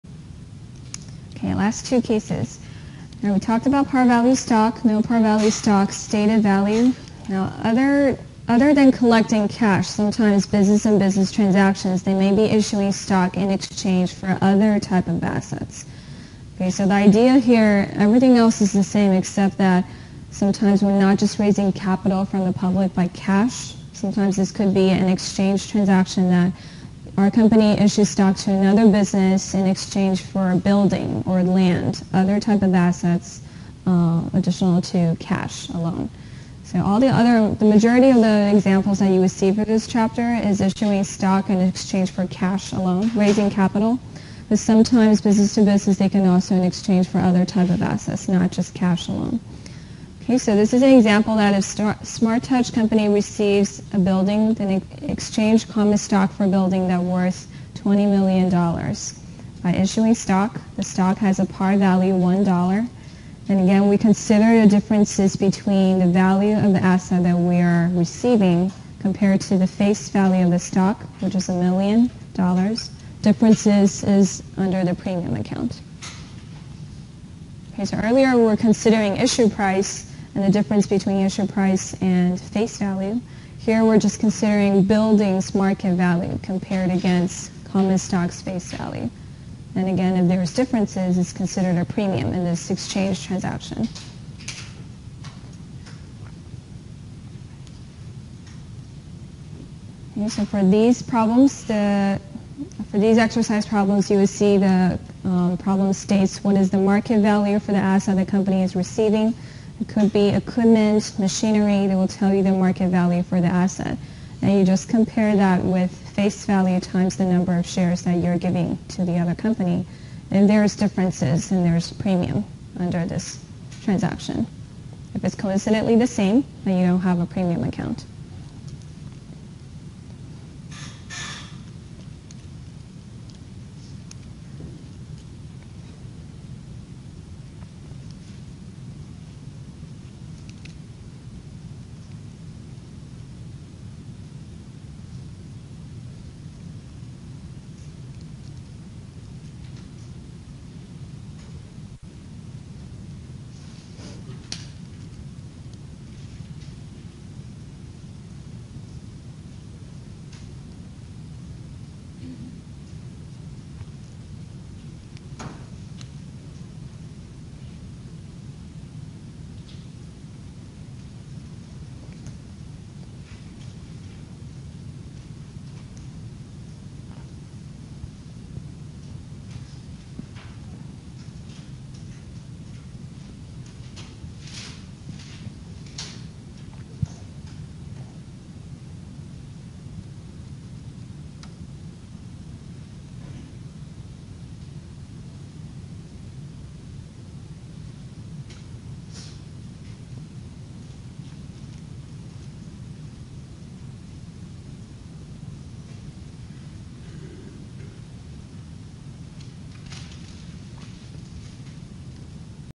Accounting for Stock Issuances - with par value, issued for non-cash assets
Introduction to Financial Accounting

0:05 Issuing stock for assets other than cash